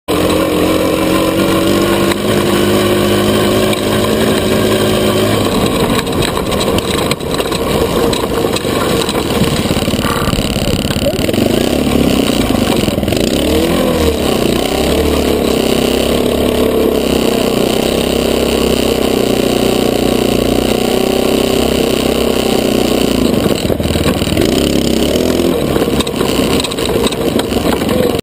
Walters Ranch April 2012 - John and I do not hit the Tree (Video 4 of 12)
Sometimes its hard to stop, but we were able to.